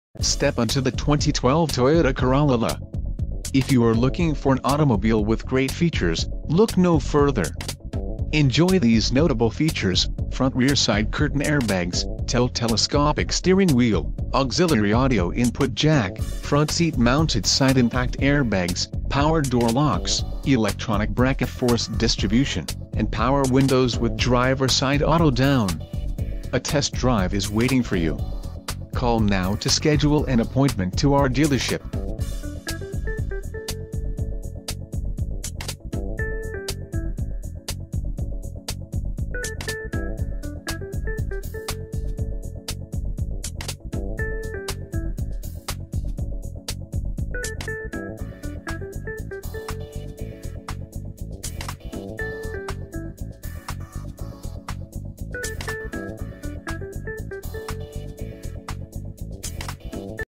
2012 TOYOTA COROLLA LE
This 2012 Toyota Corolla LE is offered exclusively by Nalley Toyota With the CARFAX Buyback Guarantee, this pre-owned vehicle comes with peace of mind, standard. The CARFAX report for this 2012 Corolla LE highlights that it's a One-Owner vehicle, and for good reason. A One-Owner vehicle gives you the feel of owning a new vehicle without paying the new car price. If you are looking for a vehicle with great styling, options and incredible fuel economy, look no further than this quality automobile. More information about the 2012 Toyota Corolla: The Toyota Corolla has a longstanding reputation for being one of the most affordable small sedans available. It's known for being reliable, inexpensive and for offering a strong list of safety features.